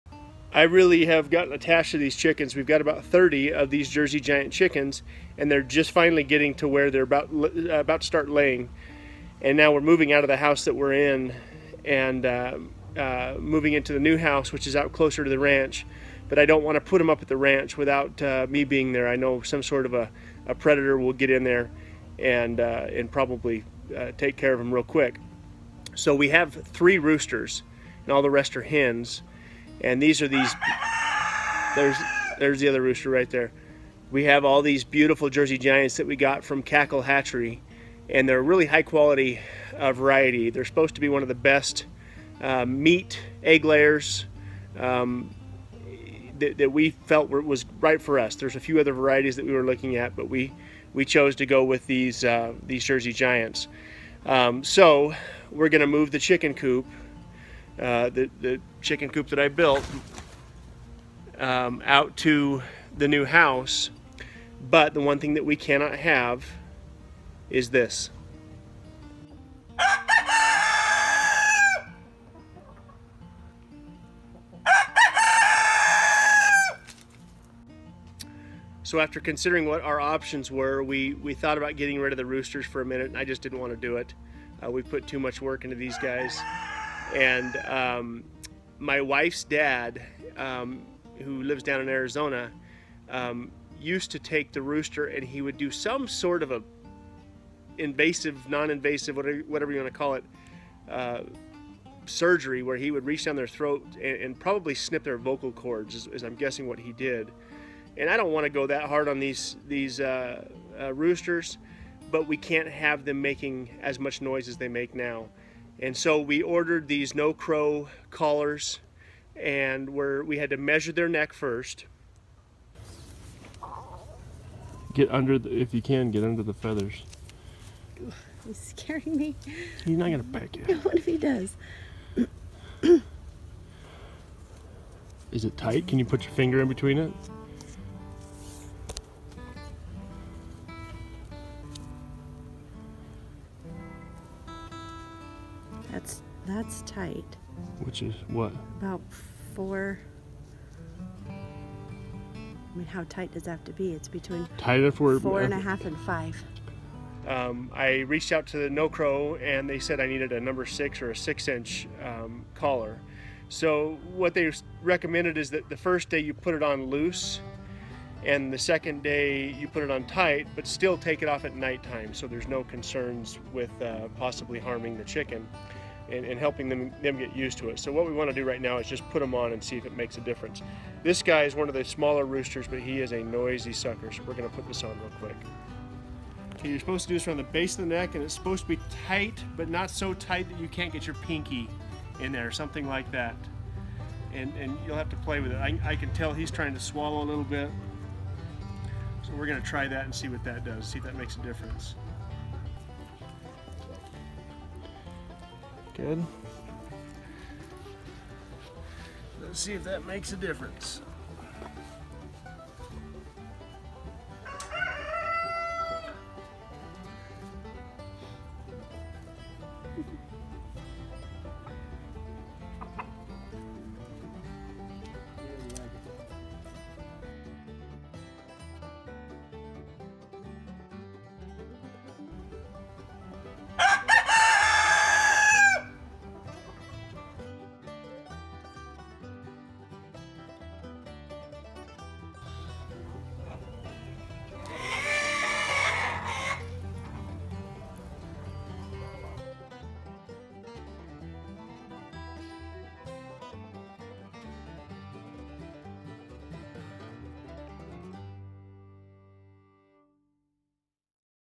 #137 - Do Rooster Collar's Really Work? (Rooster's Too Loud)
Because we are moving out to this new house we have to do something to stop the roosters from crowing. I didn't want to get rid of them so we ordered No Crow Rooster Collars. The roosters didn't like the collars at first naturally but quickly forgot about them. We woke up this morning to a lack of roosters crowing. Success.

Video about Lifevantage products and the science behind them.